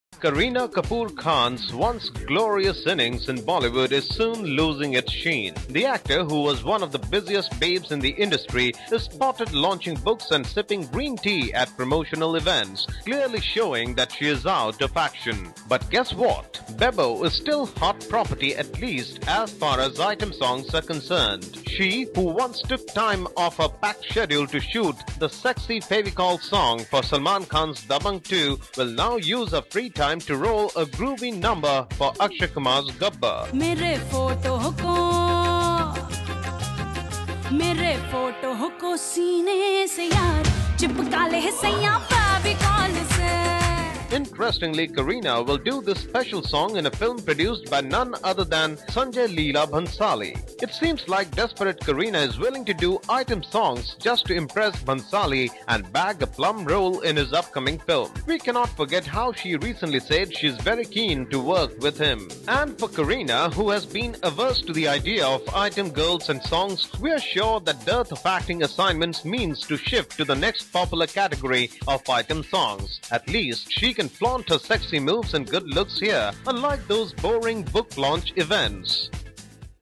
Bollywood's once hot property, Kareena Kapoor Khan is now turning to item songs
Kareena Kapoor Khan's once glorious innings in Bollywood is soon losing its sheen. The actor who was one of the busiest babes in the industry is spotted launching books and sipping green tea at promotional events, clearly showing that she's out of action.